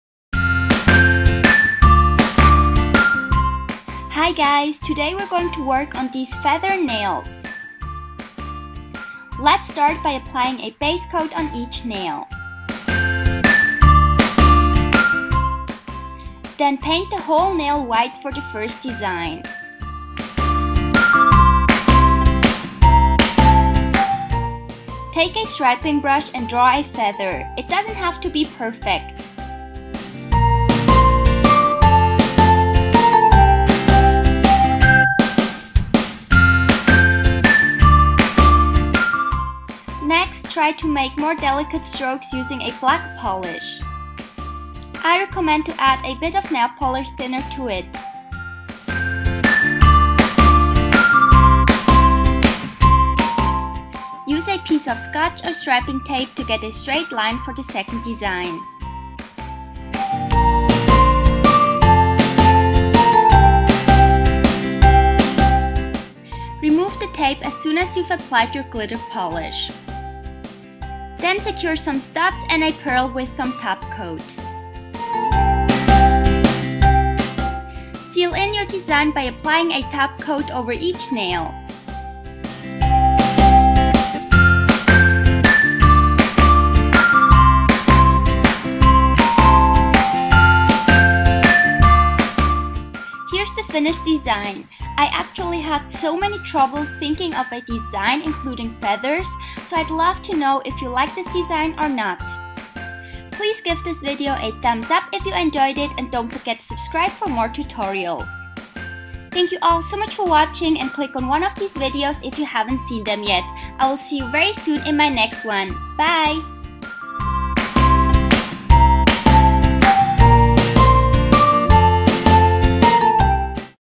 Design 10: Feathers | Feather & Glitter Nail Art ♡
Happy Thanksgiving to all of you who celebrate it! Here's a very quick tutorial which actually took my a lot of hours to do since I wasn't able to come up with a good design! Participating in a nail art challenge is truly a challenge! One more design to go! :D

xo Sarah

♡ PRODUCTS USED ♡
"Wink Beach" by Parado
"Nevermore" by Cult Nails
"Tempest by Cult Nails"
"Silhouette" by Revlon
Base and top coat